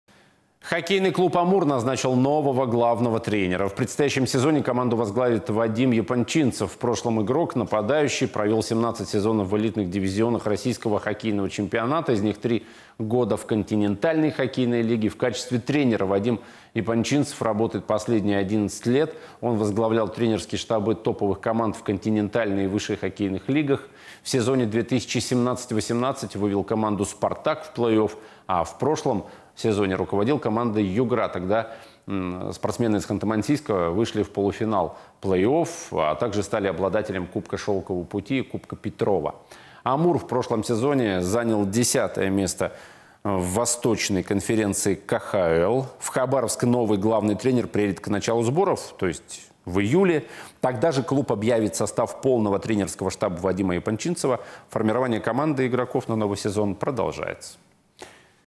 Главным тренером хоккейного клуба «Амур» назначен Вадим Епанчинцев. Новости.25/05/22
Хоккейный клуб «Амур» назначил нового главного тренера. В предстоящем сезоне команду возглавит Вадим Епанчинцев. В прошлом игрок, нападающий, он провел 17 сезонов в элитных дивизионах.